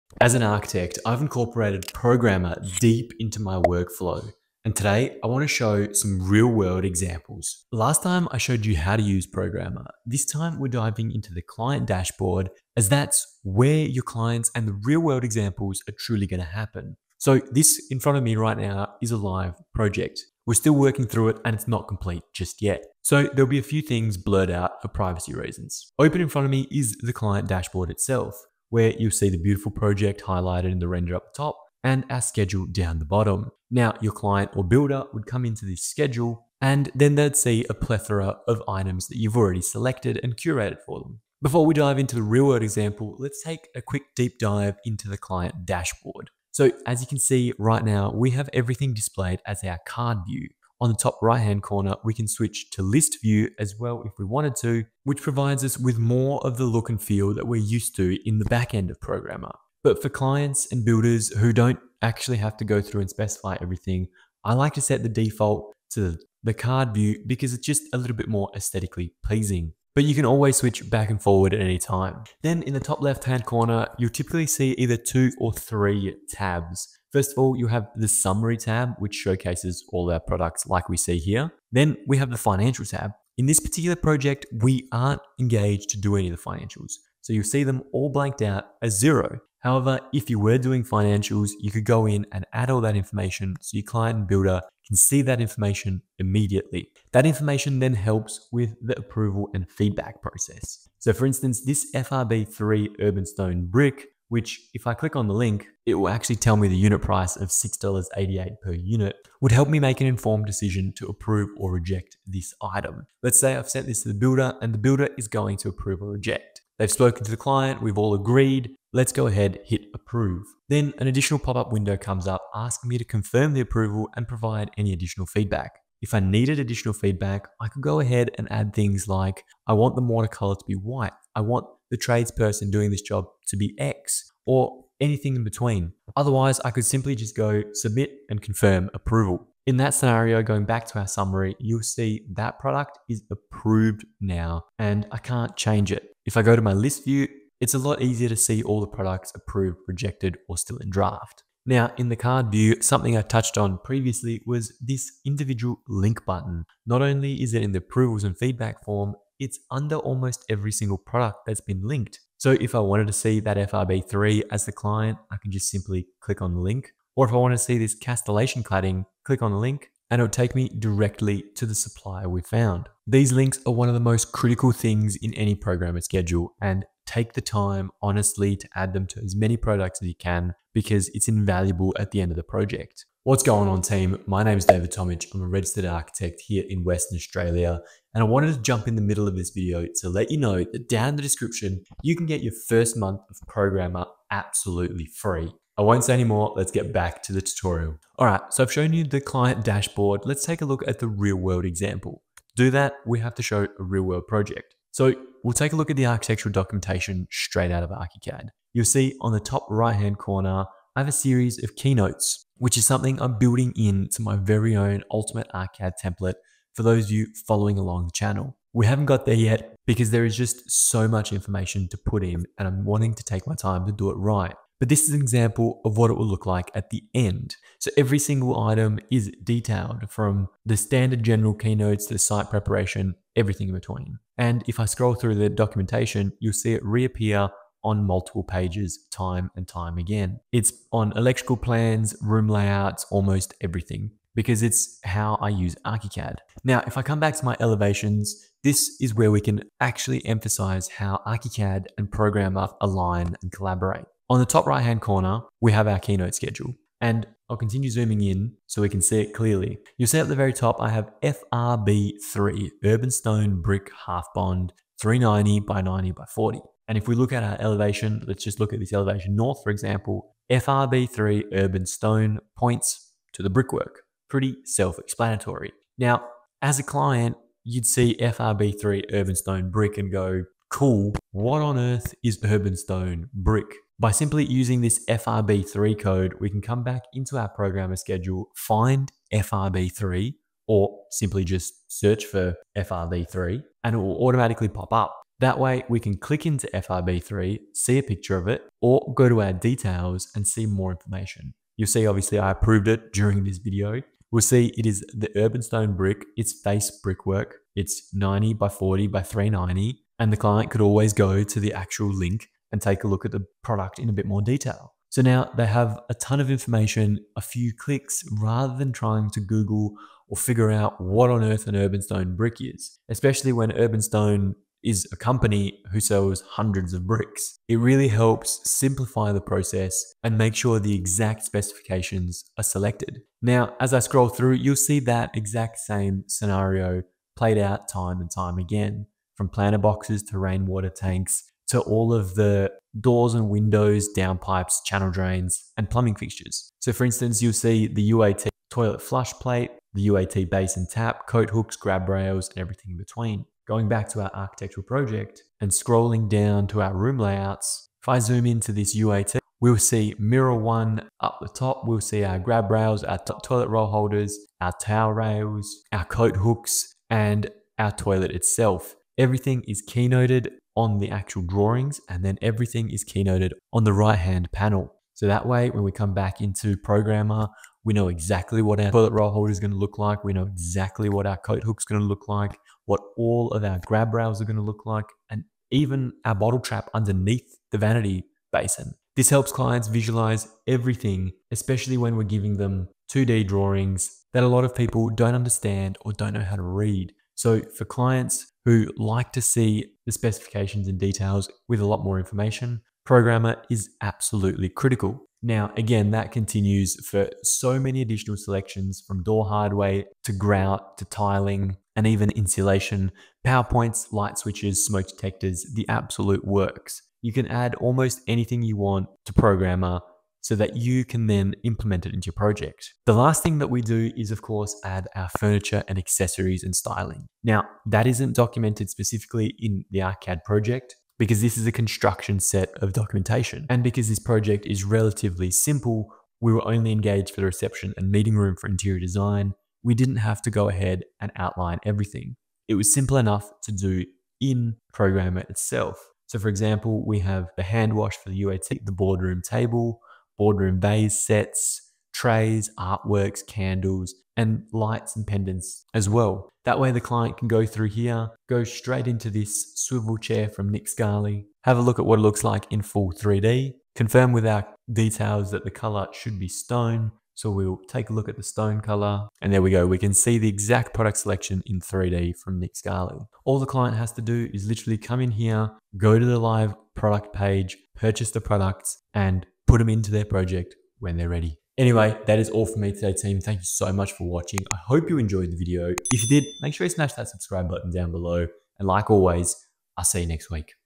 ArchiCAD and Programa The Perfect Pair
In this follow-up tutorial, I go beyond the basics of Programa to show you how I use it in real-world architecture projects — ArchiCAD + Progama the perfect pair. This isn’t just another software walkthrough — it’s how I integrate Programa into my actual workflow as a registered architect here in Western Australia.

✅ See a real project using Programa’s Client Dashboard
✅ Learn how to use material codes across ArchiCAD and Programa
✅ Streamline approvals, track feedback, and simplify spec sheets
✅ Help your clients understand complex documentation with visual cues
✅ Link mood boards, supplier data, and product links — all in one place

If you’re looking to improve communication between your clients, consultants, and builders, Programa is the tool for you.

💬 Drop any questions below — and let me know how you’re using Programa in your own practice.

🔽 Don’t Miss Out!
Timestamps:
00:00 Intro
00:41 Client Dashboard
03:02 Freebies
03:18 ArchiCAD and Programa